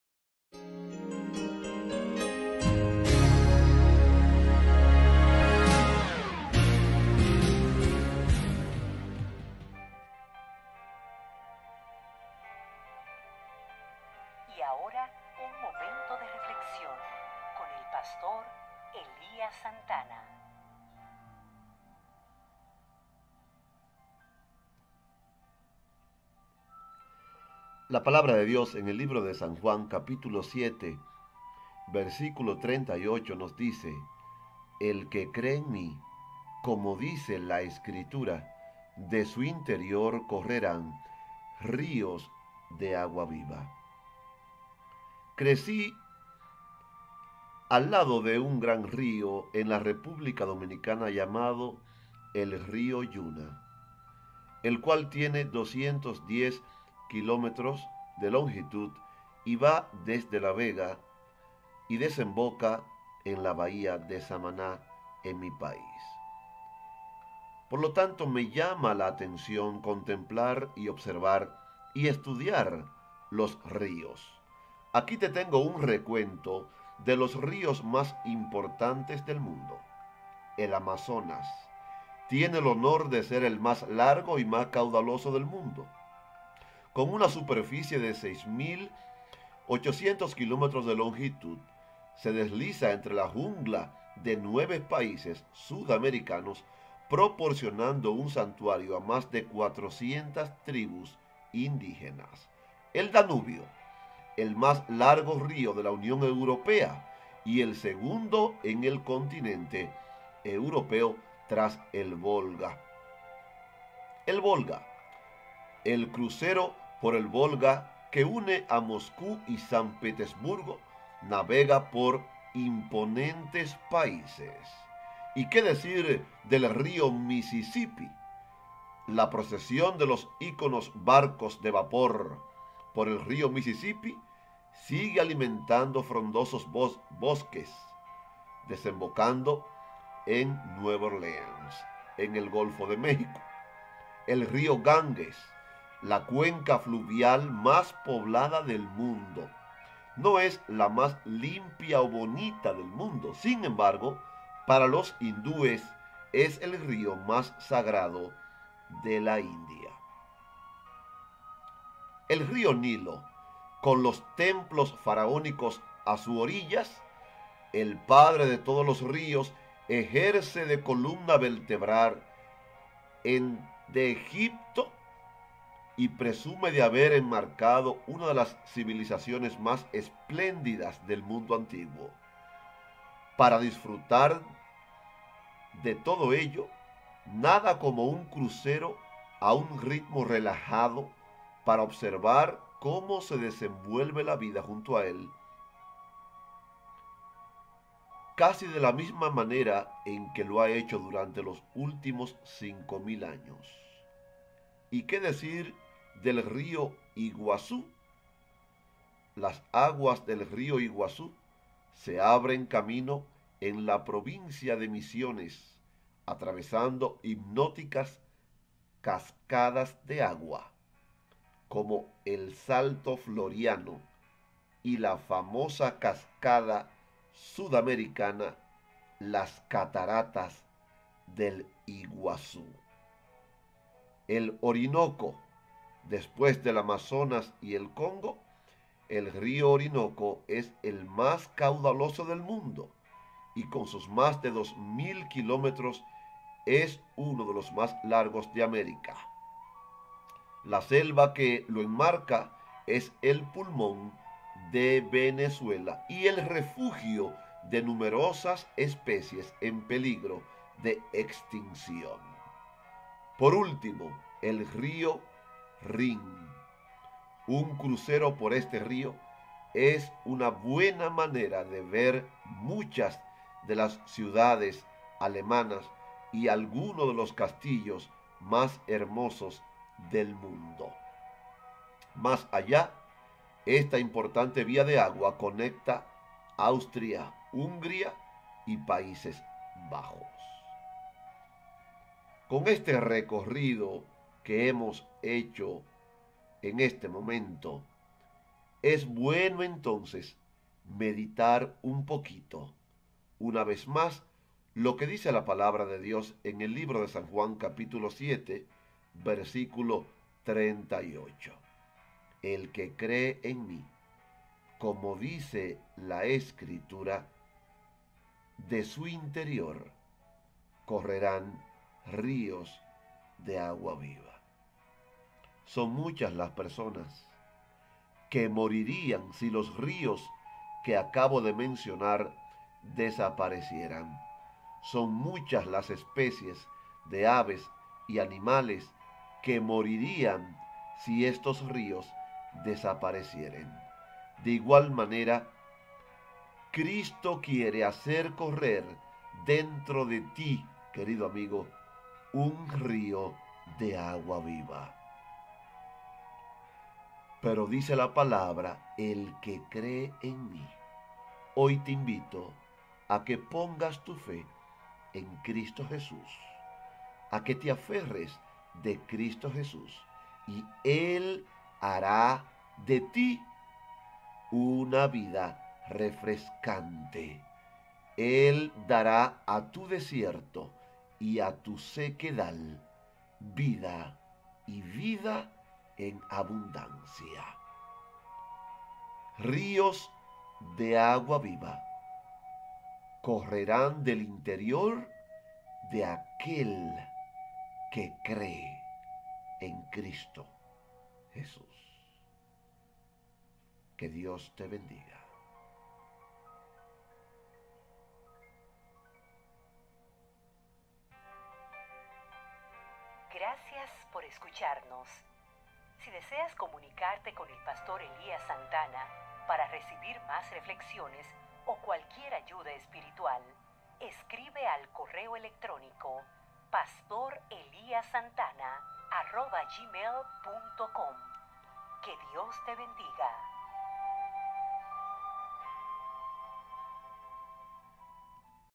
Ríos de agua viva. Juan 7:38
”Trae tu sequedal y tu desierto a Jesús y tu vida será como ríos de agua viva”